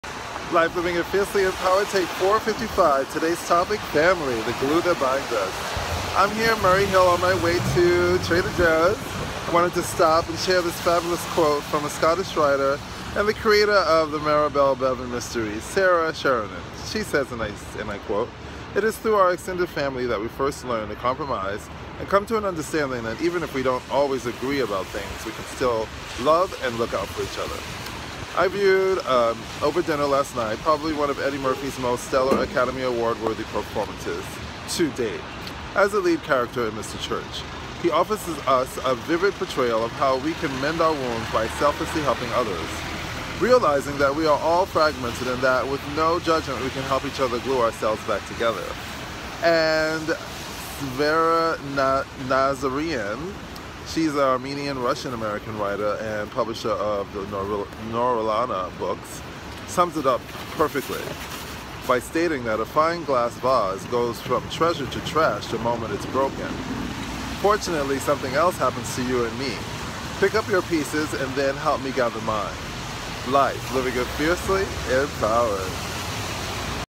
"LIFE!" take 455 (Living It Fiercely Empowered)
Blane Charles founder of BCDC (Blane Charles Design Consulting) offers you a one minute daily dose of pure inspiration inviting you to own your greatness everyday and dare to live the life of your dreams!

Tee JOE FRESH
Car Coat MAISON MARTIN MARGIELA
Hoody RICK OWENS
Scarf KENZO
KAREN WALKER EYEWEAR